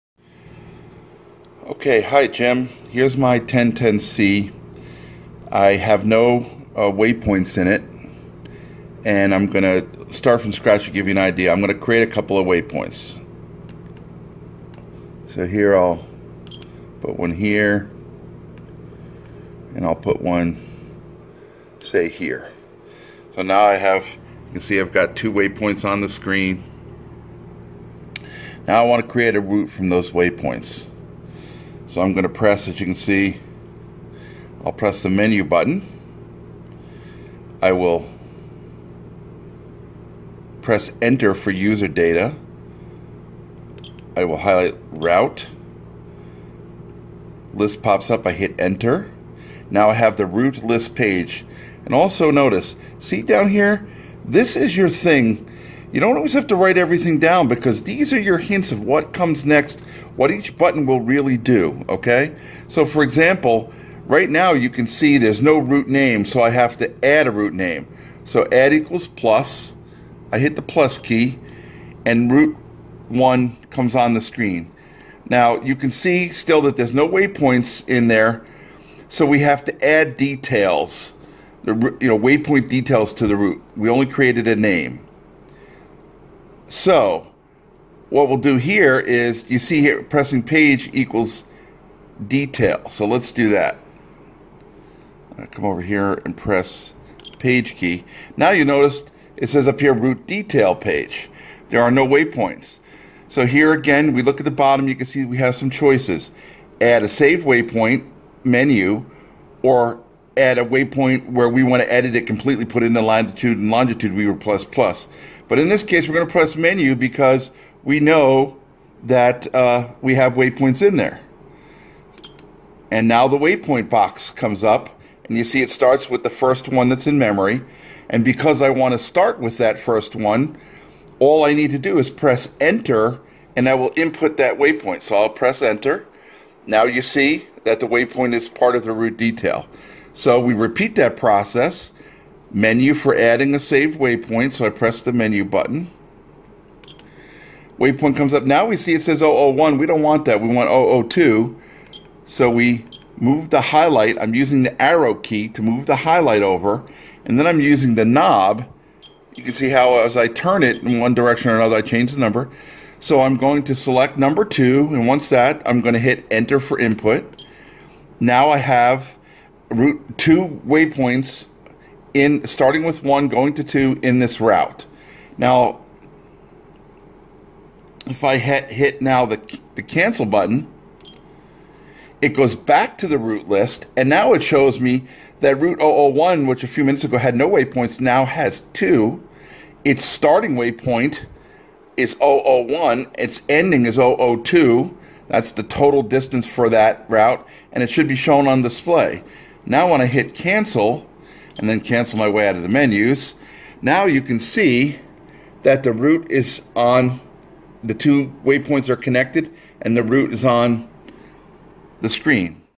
Sitex SVS Series Plotter Manual Route Creation using Existing Waypoints
A video explaining how to create a route manually using the existing way-point list on any SI-TEX SVS Series Chart Plotter.